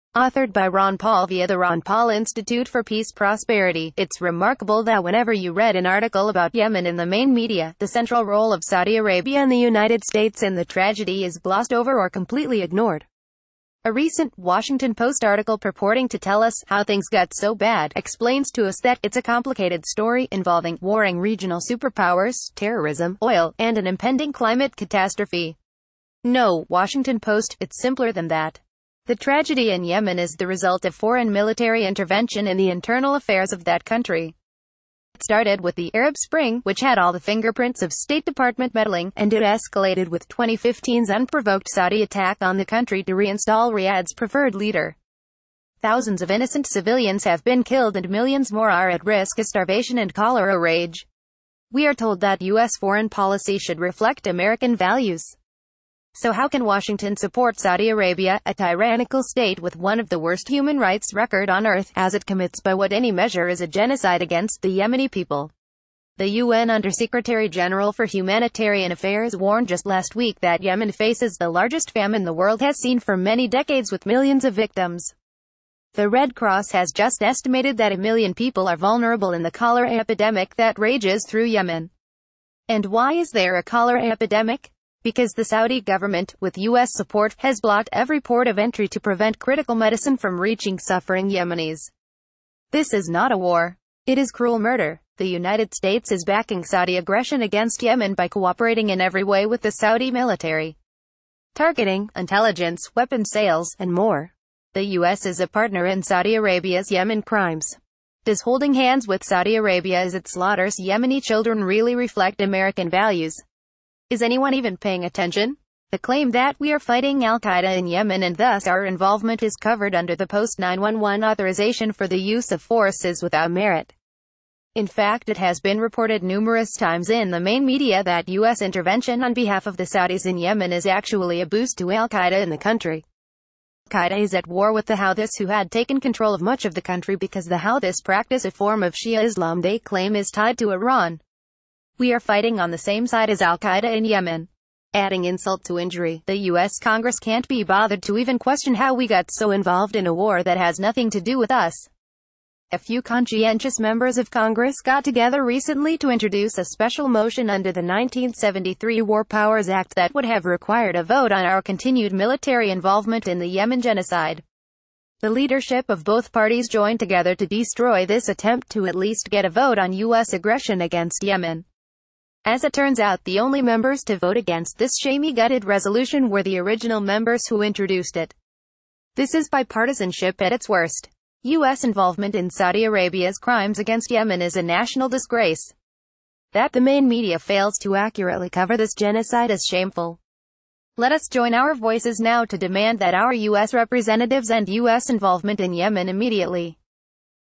TOP NEWS - Ron paul asked why we are helping to destroy yemen saudi arabia?
TOP NEWS HEADLINES DAILY - TOP NEWS - Ron paul asked why we are helping to destroy yemen saudi arabia?

Authored by Ron Paul via The Ron Paul Institute for Peace Prosperity, It’s remarkable that whenever you read an article about Yemen in the main media, the central role of Saudi Arabia and the United States in the tragedy is glossed over or completely ignored. A recent Washington Post article purporting to tell us “how things got so bad” explains to us that, “its a complicated story” involving “warring regional superpowers, terrorism, oil, and an impending climate catastrophe.” No, Washington Pos...